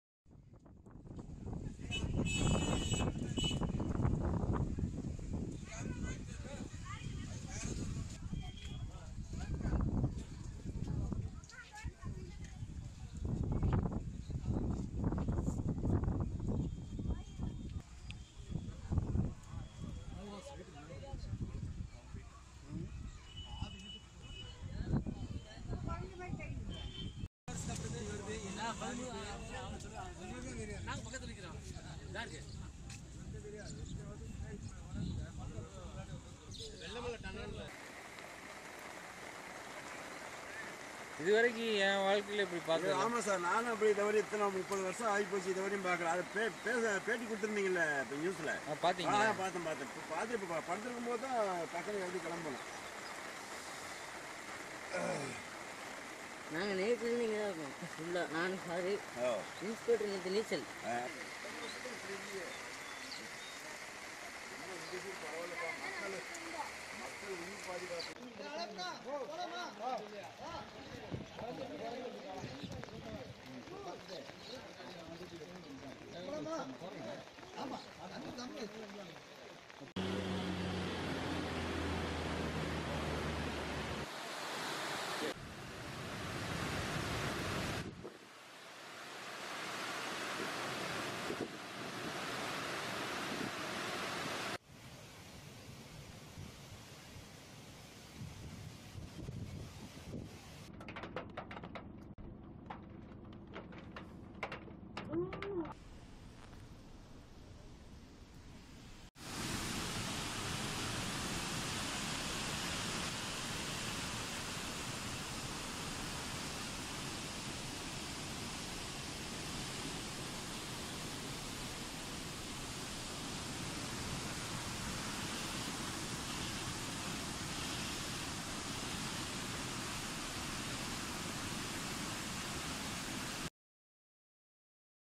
கனமழை: உருண்டோடும் வெள்ளம்; தத்தளிக்கும் வால்பாறை
வால்பாறையில் விடியவிடிய பெய்த கனமழையின் காரணமாக பல வீடுகள் நீரில் மூழ்கியுள்ளதால், மக்கள் தனியார் காட்டேஜ்கள், மண்டபங்கள், பள்ளிகளில் தங்க வைக்கப்பட்டுள்ளனர்.